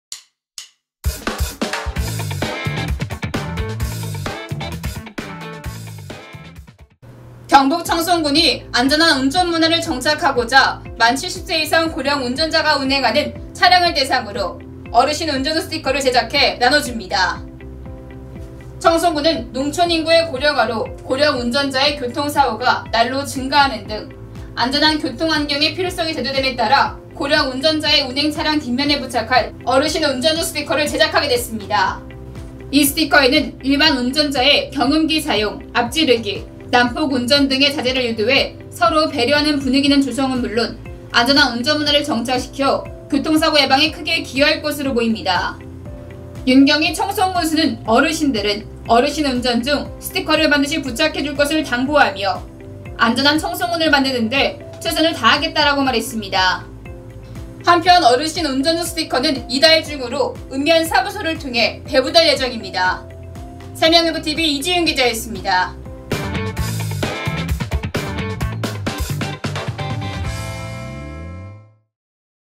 청송군 윤경희군수 고령운전자 어르신 운전중 스티커 제작 배부 세명일보 세명일보TV
청송군(군수 윤경희)이 안전한 운전문화를 정착하고자 만70세 이상 고령운전자가 운행하는 차량을 대상으로 ‘어르신 운전중’ 스티커를 제작해 나눠준다.

군은 농촌인구의 고령화로 고령운전자의 교통사고가 날로 증가하는 등 안전한 교통 환경의 필요성이 대두됨에 따라 고령운전자의 운행 차량 뒷면에 부착할 ‘어르신 운전중’ 스티커를 제작하게 됐다.

이 스티커는 일반운전자의 경음기 사용·앞지르기·난폭운전 등의 자제를 유도해 서로 배려하는 분위기 조성은 물론, 안전한 운전문화를 정착시켜 교통사고 예방에 크게 기여할 것으로 보인다.

윤경희 청송군수는 “어르신들은 ‘어르신 운전중’ 스티커를 반드시 부착해 줄 것을 당부하며 안전한 청송군을 만드는데 최선을 다하겠다”고 말했다.

한편 ‘어르신 운전중’ 스티커는 이달 중으로 읍·면사무소를 통해 배부될 예정이다.